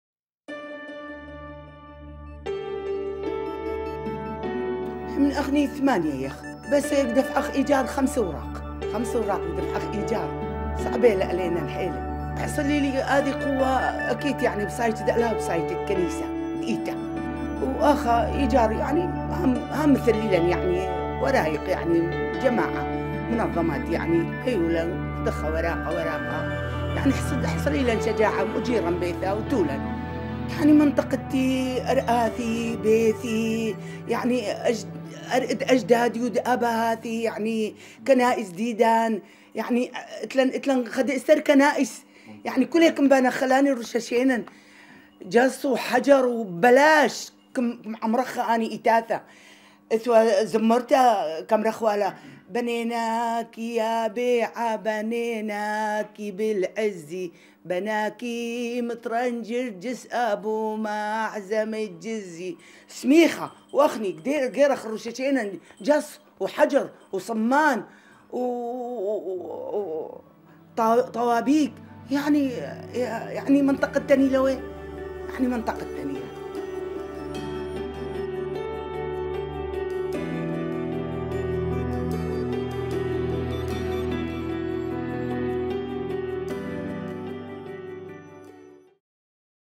Iraqi grandmother wants to go back home (German subtitles)
This is a short testimony of an Iraqi grandmother who wants to go back to her home in Qaraqosh in the Nineveh Plains in Iraq.